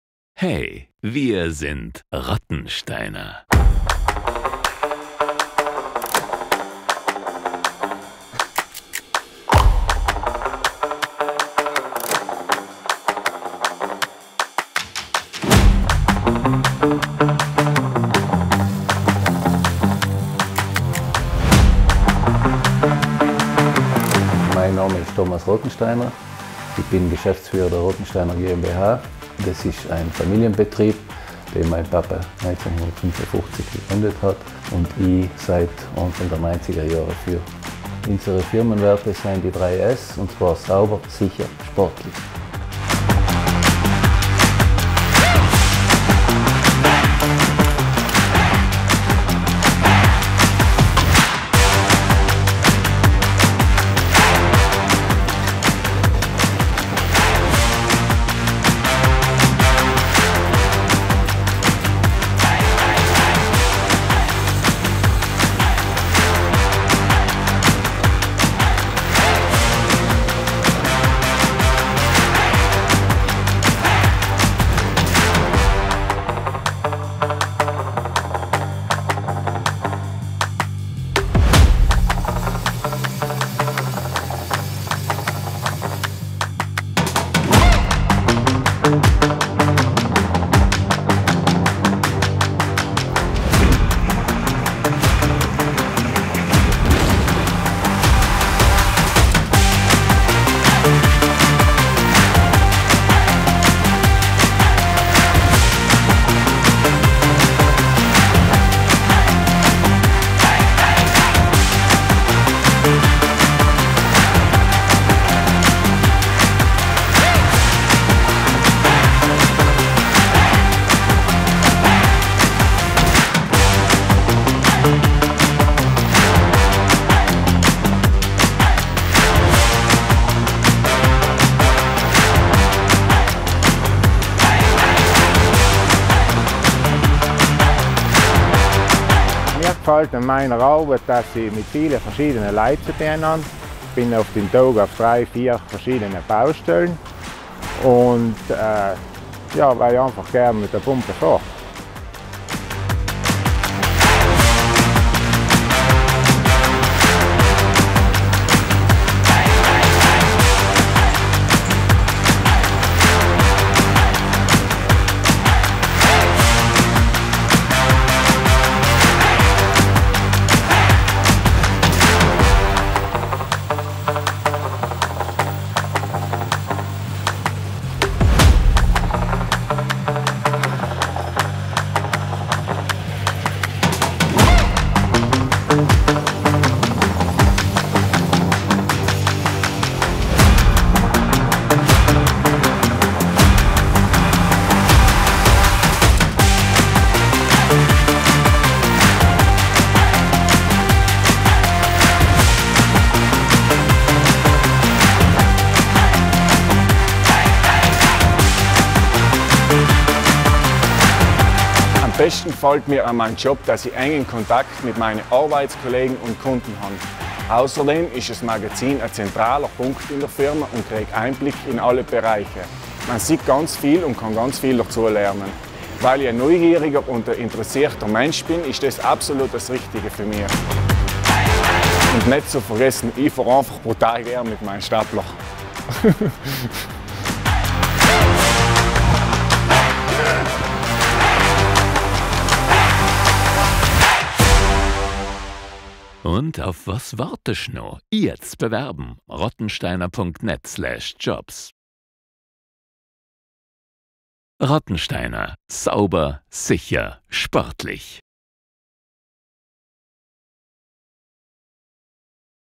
Wir sind rottensteiner
Wir bewegen Großes im Tiefbau und bauen auch für deine Zukunft. Vielfältige Leistungen rund um den Bau bewältigen wir mit den drei S: sauber, sicher und sportlich. Wir sind ein verlässlicher Familienbetrieb am Ritten/Südtirol und ein kollegiales Team. Innovativ und bodenständig zugleich. Wir lieben große Maschinen und kleine Details. Du suchst deinen Traumjob? Dann bewirb dich jetzt bei uns!